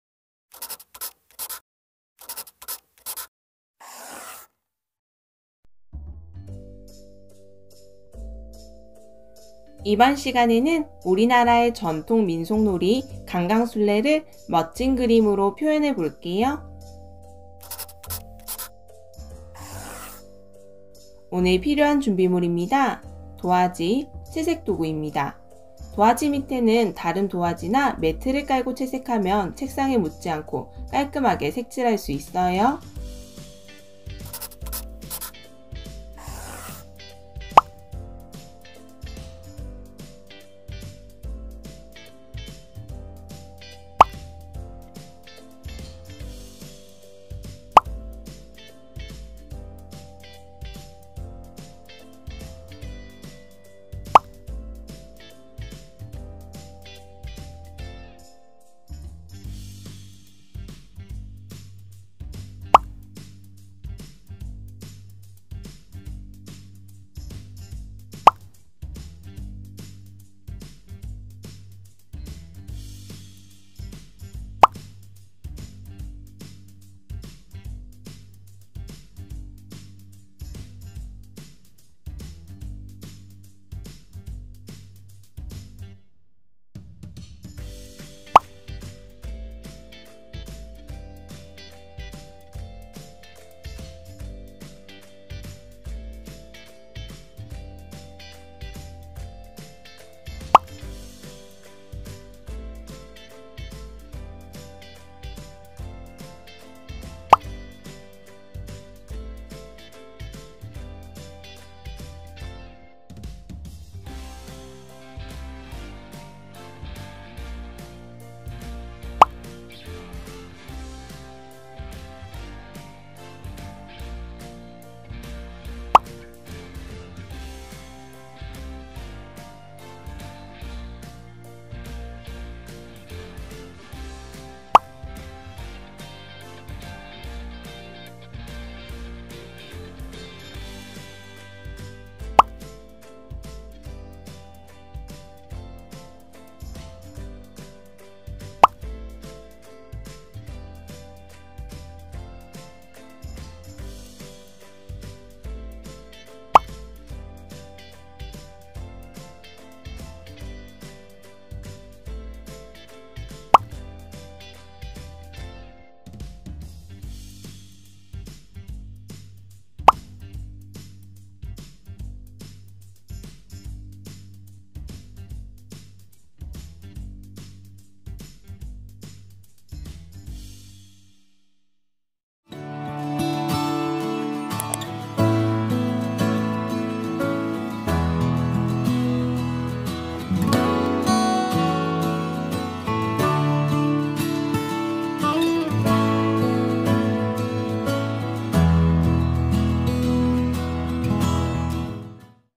442 강강술래 그리기 _ 추석미술
영상을 일시정지 하고 따라 그려요.

저고리, 치마 색을 알록달록 다양하게 해도 좋아요.